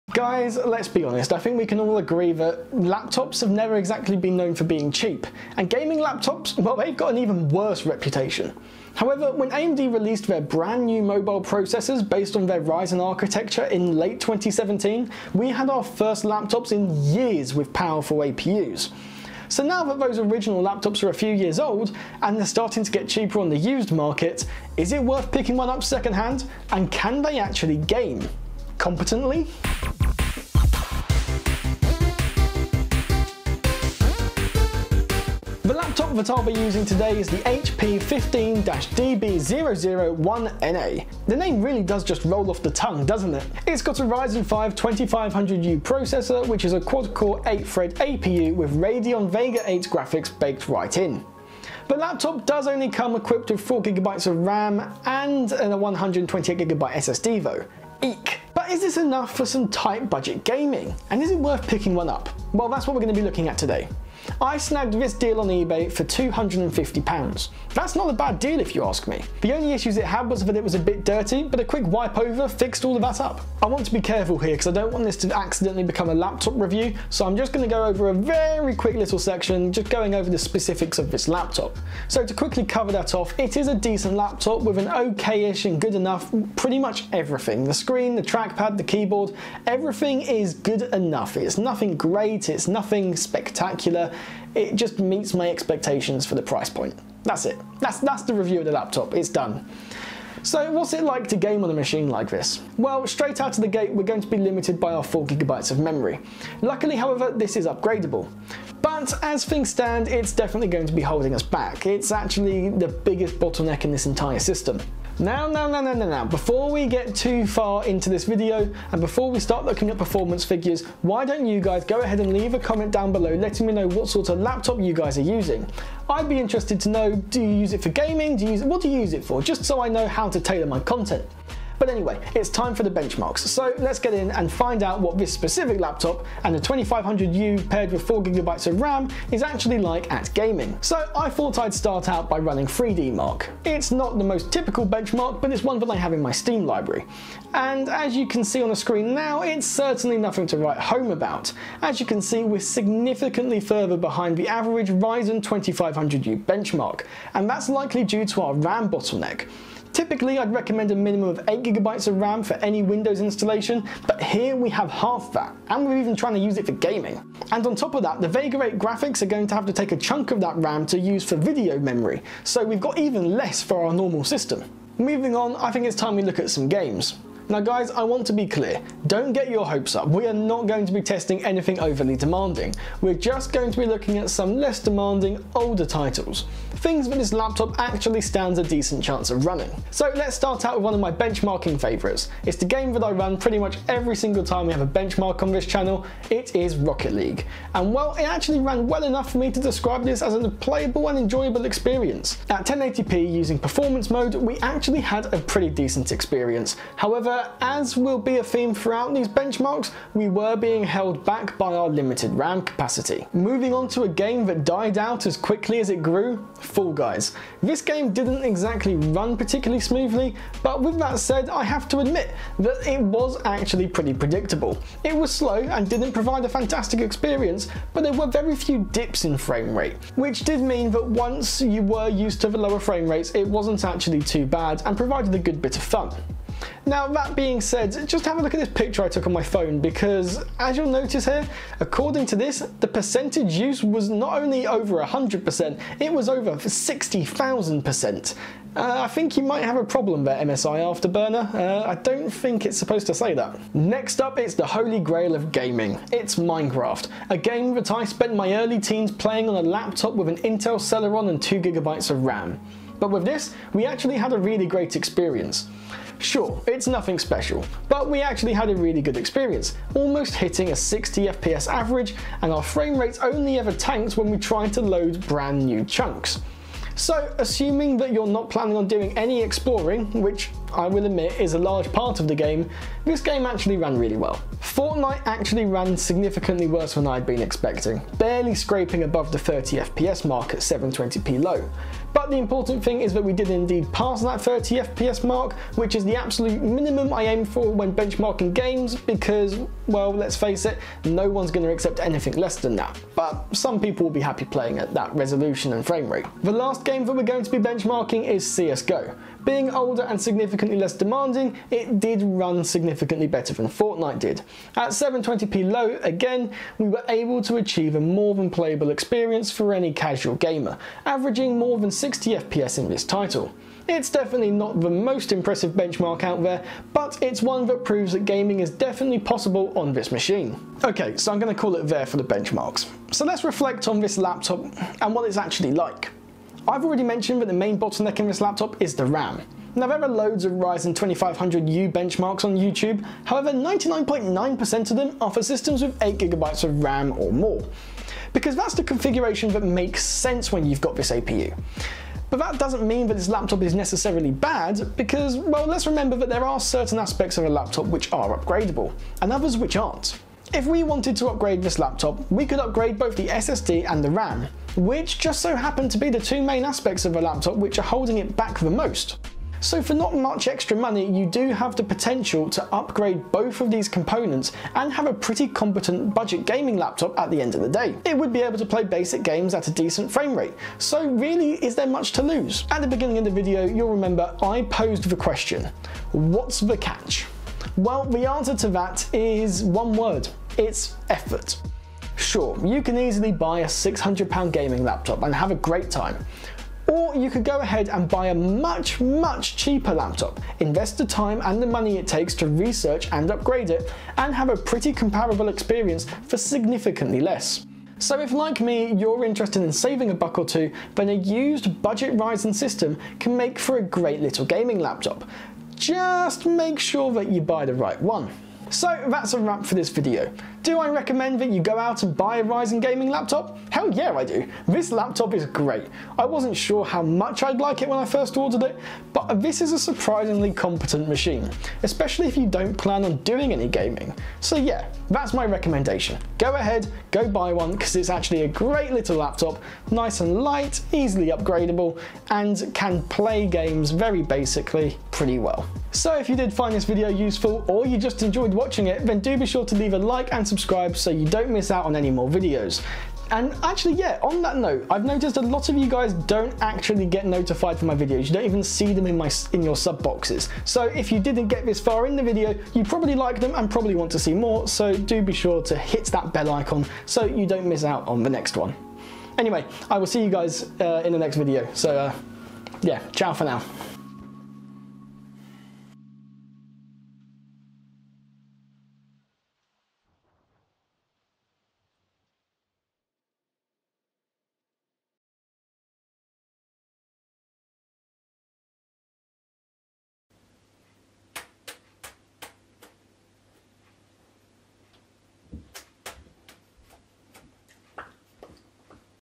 Gaming on a CHEAP RYZEN Laptop - But What's the Catch?
Why waste hundreds of dollars on an expensive gaming laptop when the same experience can be had for less... Or can it? Today we'll be discussing the trade-offs of purchasing a cheap Ryzen Gaming Laptop under £250/$350.

SUPPORT RMDTECH:
Purchase Featured Product
Amazon Storefront